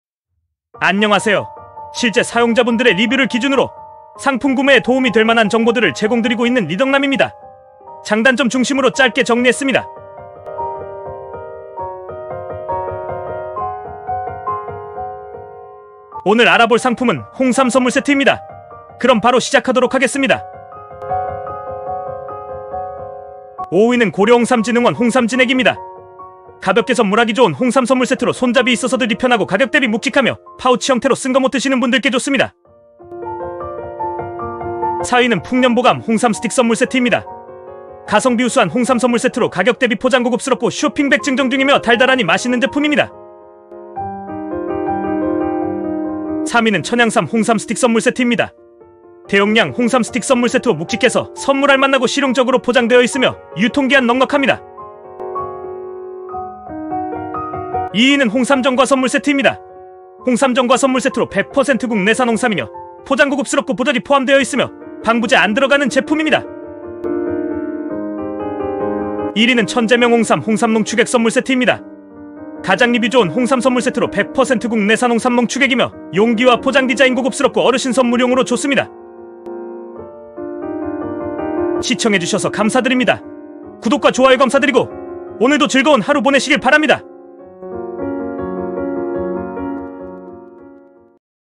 리뷰 좋은 홍삼선물세트 추천 2024 Best 5
구매링크는 아래에 [자세히 보기]를 누르시면 보입니다. 감사합니다.
0:25
5위
고려홍삼진흥원 홍삼진액 (1만 2천원)

0:37
4위
풍년보감 홍삼스틱선물세트 (1만 5천원)

0:48
3위
천양삼 홍삼스틱선물세트 (4만 6천원)

0:59
2위
홍삼정과 선물세트 (4만 7천원)

1:11
1위
천제명홍삼 홍삼농축액선물세트 (7만 4천원)